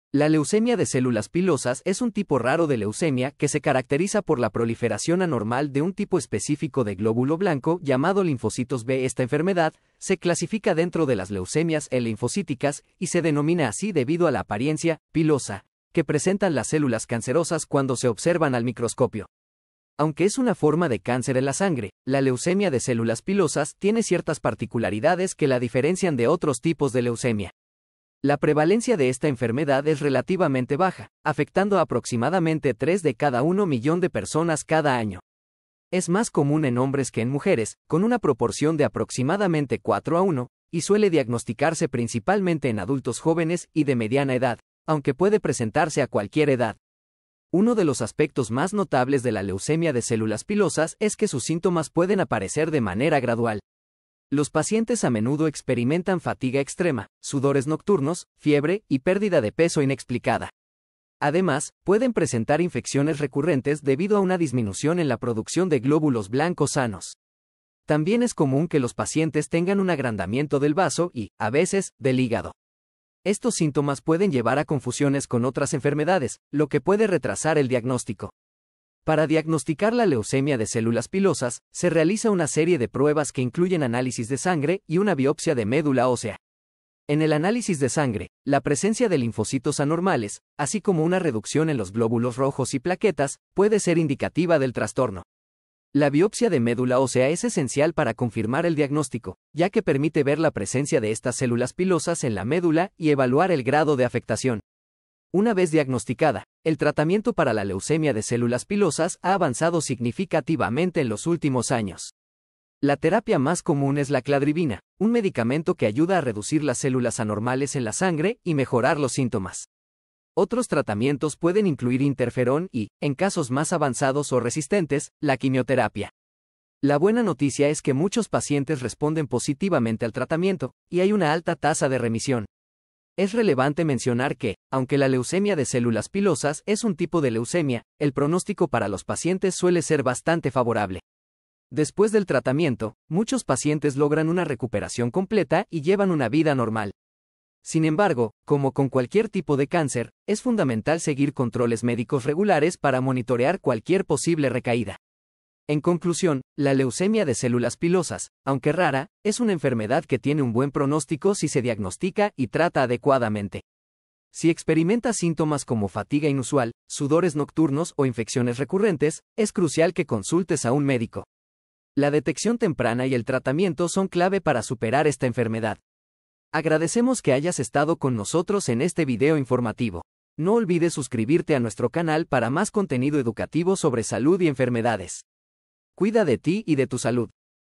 Leucemia de células pilosas Video Informativo Español
Información general sobre una condición medica. Contenido creado utilizando inteligencia artificial con el objetivo de conectar el acceso entre comunidades. Por favor, señale cualquier sugerencia o error en la sección de comentarios. ¡Gracias!